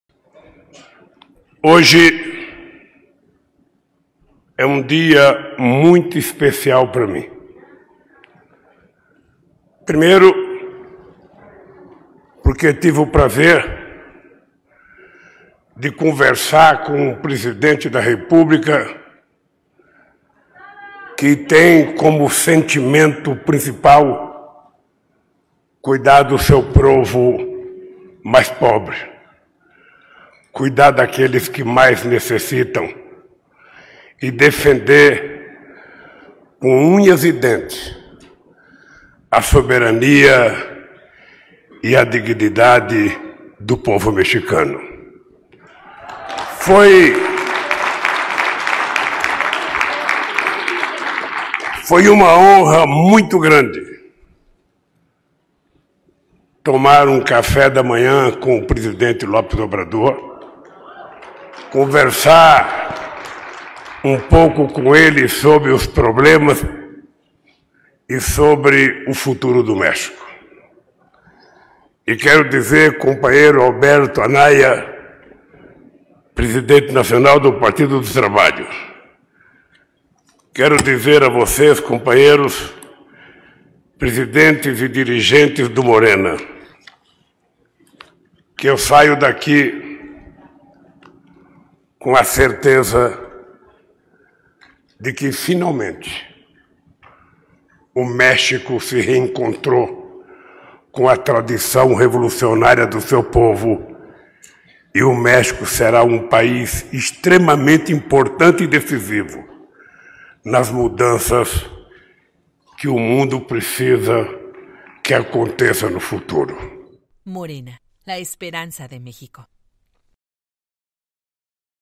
Mensaje de Luiz Inácio Lula da Silva
"...finalmente México se reencontró con la tradición revolucionaria de su pueblo (...)".

- Luiz Inácio Lula da Silva
Expresidente de Brasil